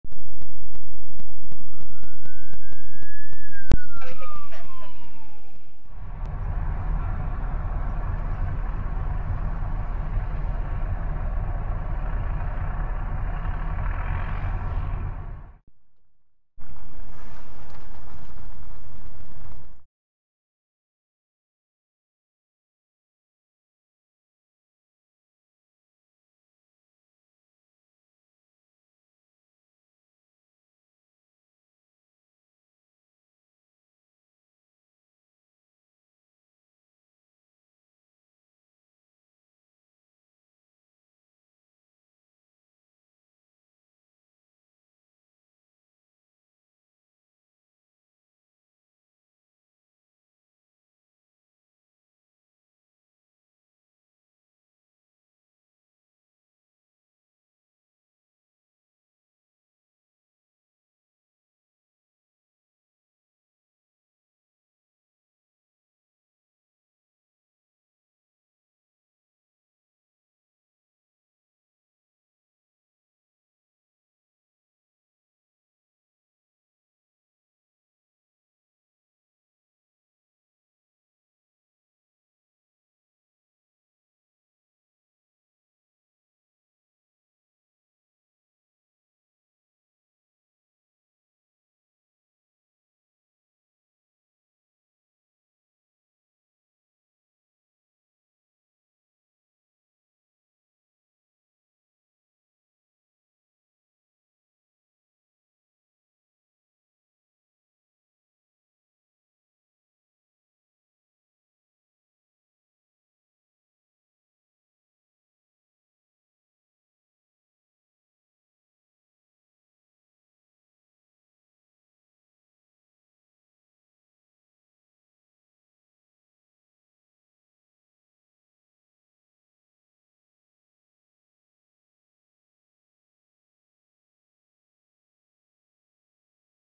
Highway 6 MVA Lavington sends two to hospital with serious injuries April 11 2023
Highway 6 was closed for a time as emergency crews dealt with a serious accident on Tuesday afternoon. Lavington and Coldstream Fire Hall were joined by two ambulances and the RCMP between Warren and Buchanan roads on Hwy 6 around 5 PM. It's not known at this time how many vehicles were involved or how the injuries were sustained.